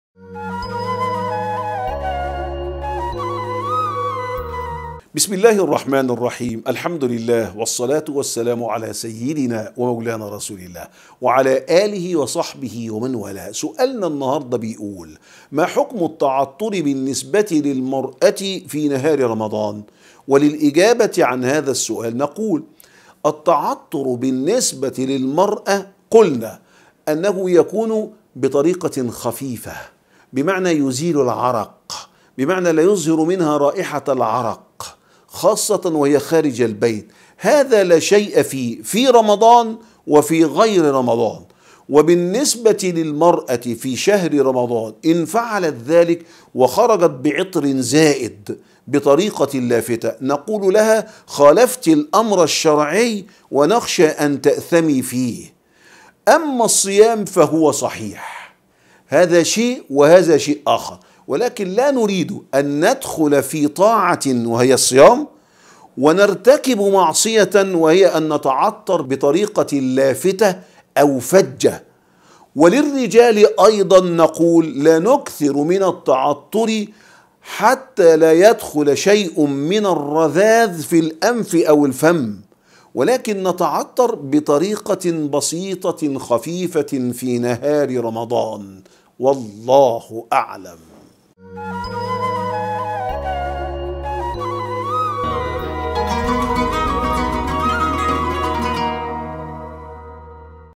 دقيقة رمضانية.. ما حكم تعطر المرأة فى نهار رمضان؟
قال الدكتور مجدى عاشور، المستشار العلمى لمفتى الجمهورية، إن تعطر المرأة فى نهار رمضان، يجب أن يكون برائحة خفيفة بمعنى أن يزيل العرق.
تابع موقعنا لأحدث الاخبار و التطورات